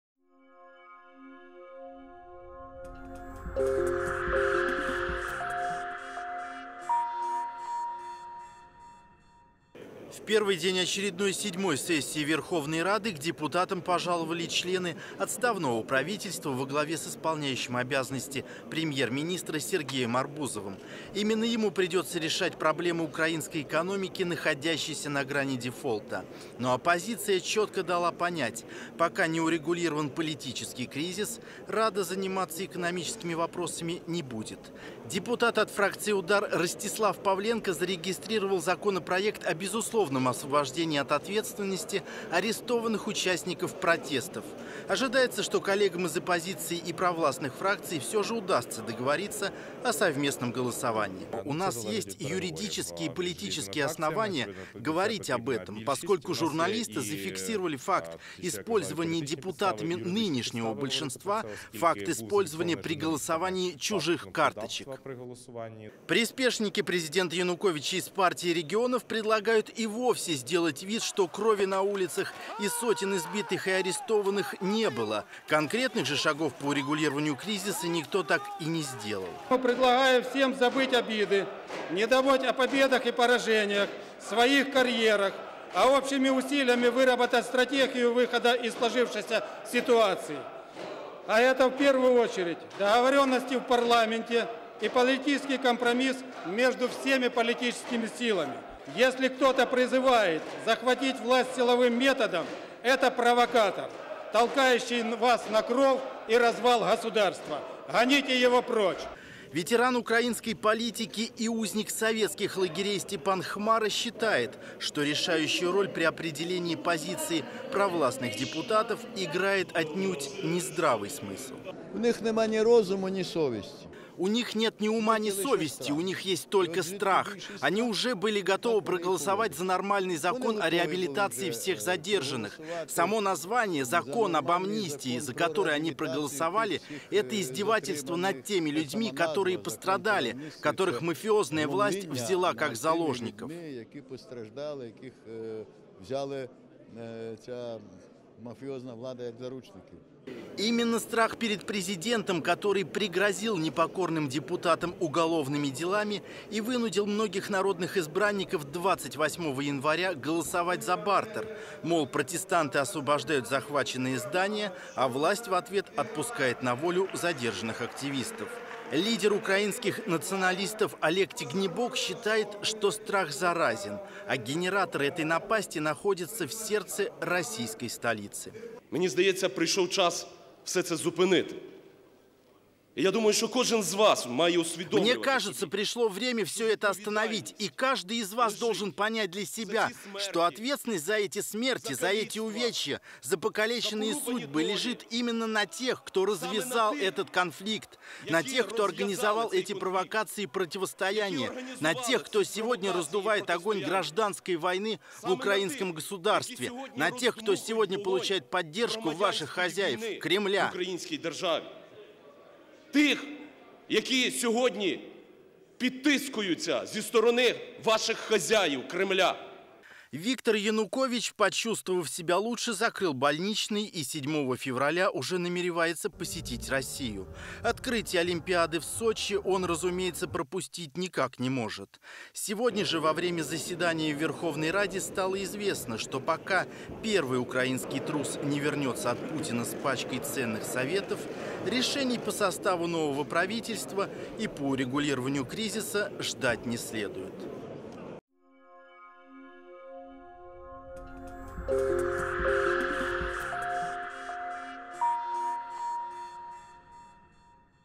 Диагноз Виктора Януковича - страх / 1612
Украинская Верховная Рада пытается урегулировать гражданское противостояние, которое длится уже два с половиной месяца. Провластные и оппозиционные депутаты заявили о необходимости разрешить конфликт, но как это сделать пока никто не знает. Оппозиционеры считают, что с оппонентами в парламенте особо говорить не о чем -- все решения принимаются в администрации президента. Из ближайшего окружения Виктора Януковича поступила информация о том, что никаких решений не стоит ожидать до возвращения украинского президента из России, куда он планирует отправиться 7 февраля